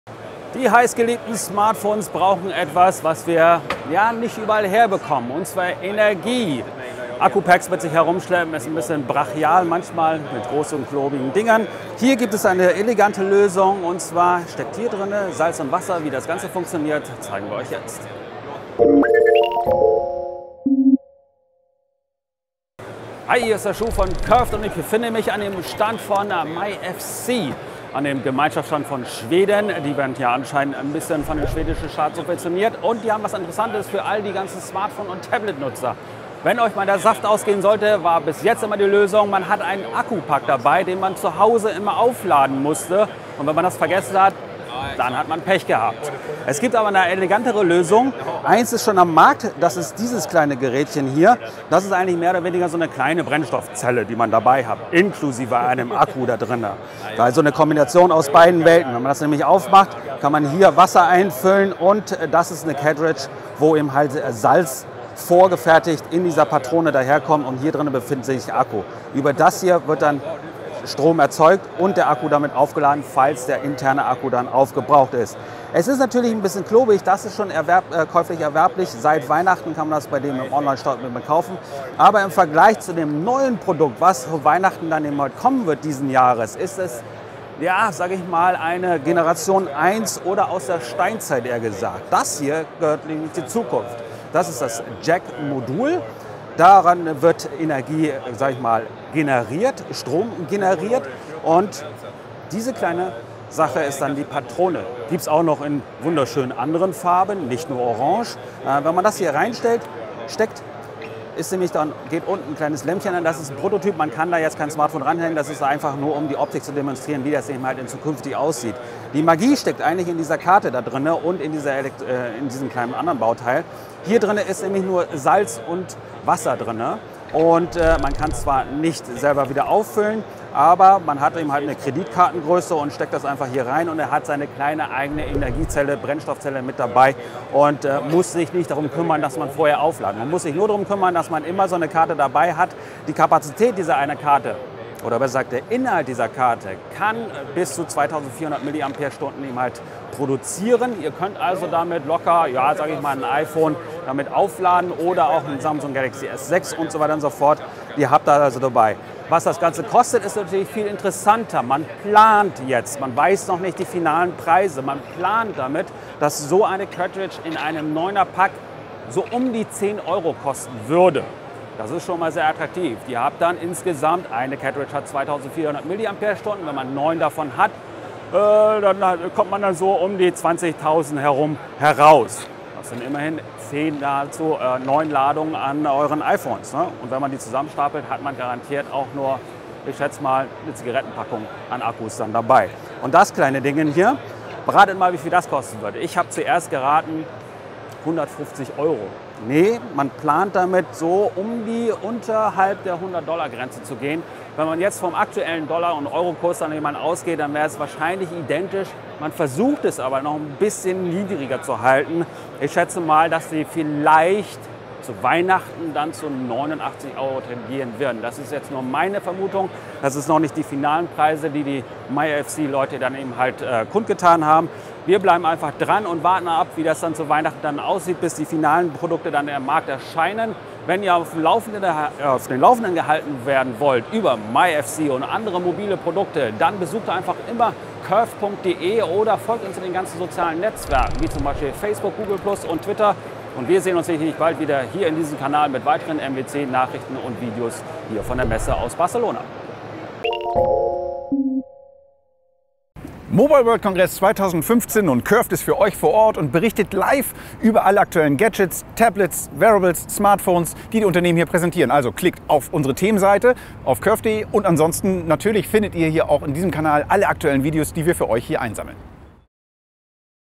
myFC JAQ im Hands-On | deutsch | MWC
Statt eines schweren Akkus, den man dann auch noch jeden Tag aufladen muss, kann man sich schon zu Weihnachten 2015 das myFC JAQ anschaffen und damit sein Smartphone und Tablet aufladen. Was das myFC JAQ für eine Kapazität hat und wann es auf den Markt erscheint, erfahrt ihr auf CURVED